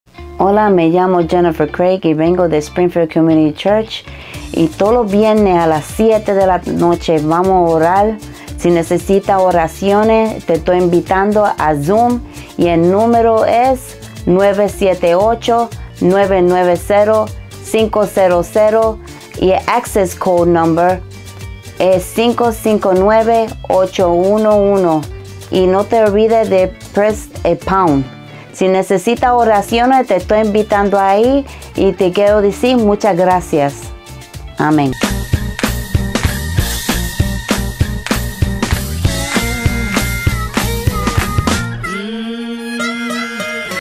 Spanish Wednesday Prayer Night
Únete a la primera dama y sus amigos todos los miércoles por la noche a las 7 pm para una noche de oración, donde abordaremos los desafíos de la vida y hablaremos en contra de todo espíritu demoníaco que intente sacudir la fe de los creyentes. En esta llamada de oración semanal completamente en español, la primera dama Jennifer Craig y un invitado especial estarán orando en tu nombre para abordar los desafíos que puedas estar enfrentando en tu vida diaria.